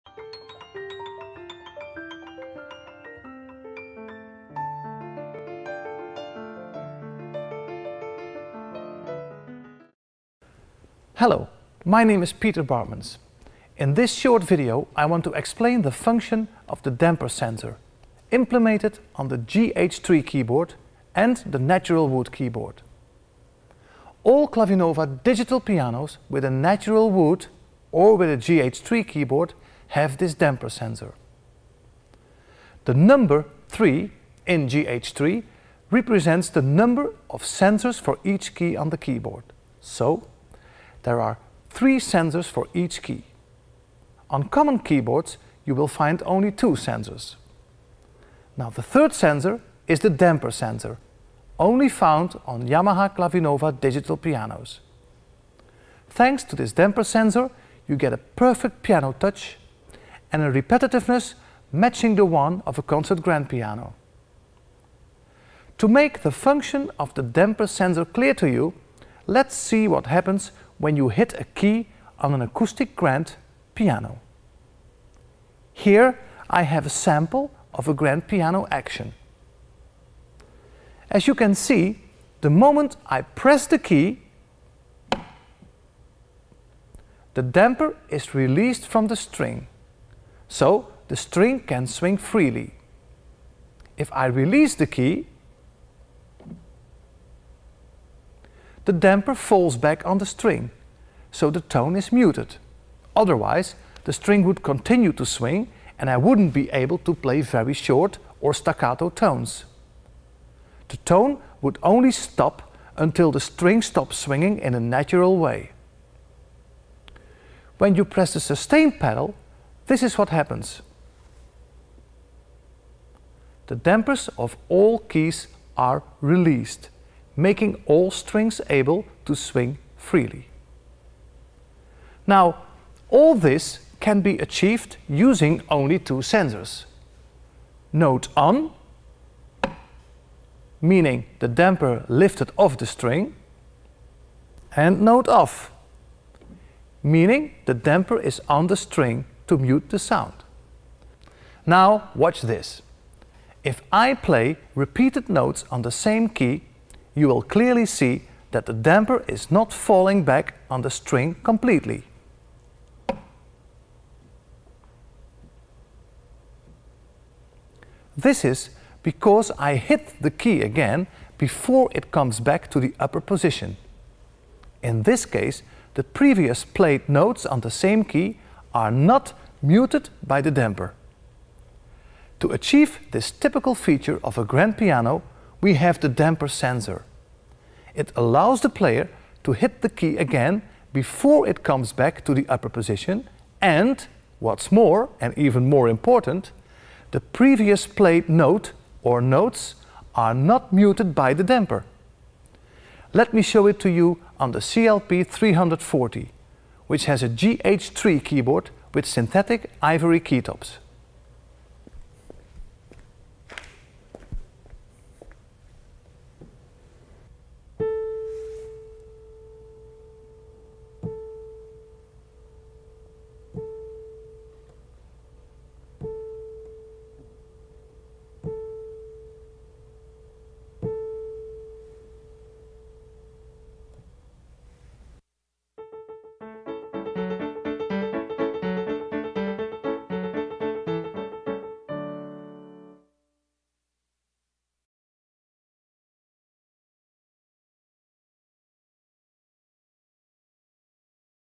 Mastery of Feel - Damper Sensor
With the ideal feel between fingers and keyboard, you can transform your intentions directly into sound.